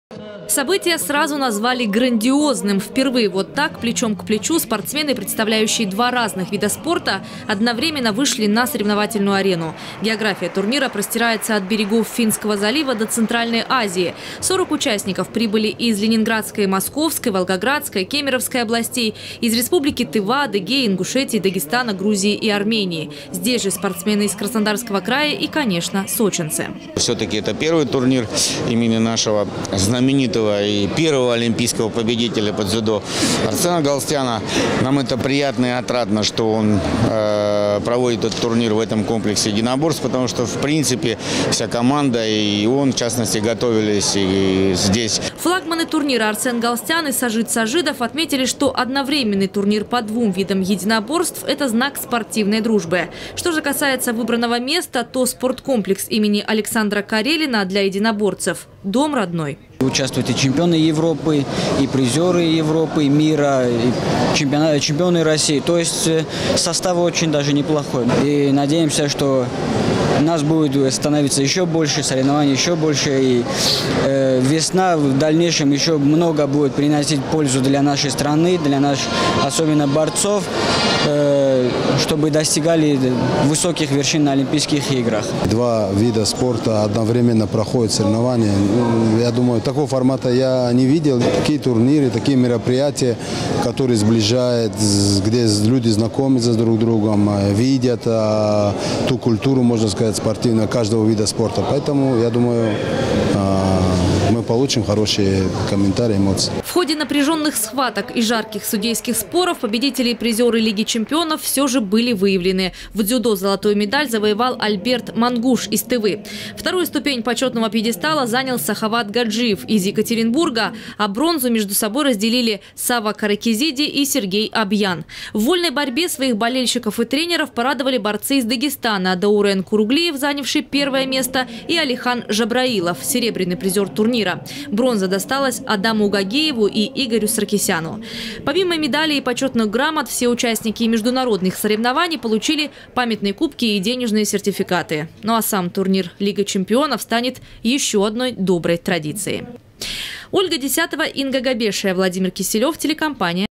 Впервые в Сочи в рамках одного турнира одновременно состязались вольники и дзюдоисты. Новости Эфкате
Открытый международный турнир «Лига Чемпионов» на призы чемпиона мира Сажида Сажидова и олимпийского чемпиона Арсена Галстяна собрал 40 взрослых и уже титулованных спортсменов.

Событие сразу назвали грандиозным. Впервые вот так, плечом к плечу, спортсмены представляющие два разных вида спорта одновременно вышли на соревновательную арену. География турнира простирается от берегов Финского залива до центральной Азии. 40 участников прибыли из Ленинградской и Московской, Волгоградской, Кемеровской, областей, ю из республики Тыва, Адыгеи, Ингушетии, Дагестана, Грузии, Армении. Здесь же спортсмены из Краснодарского края и, конечно, сочинцы.

Сочи. Новости.
Телеканал Эфкате.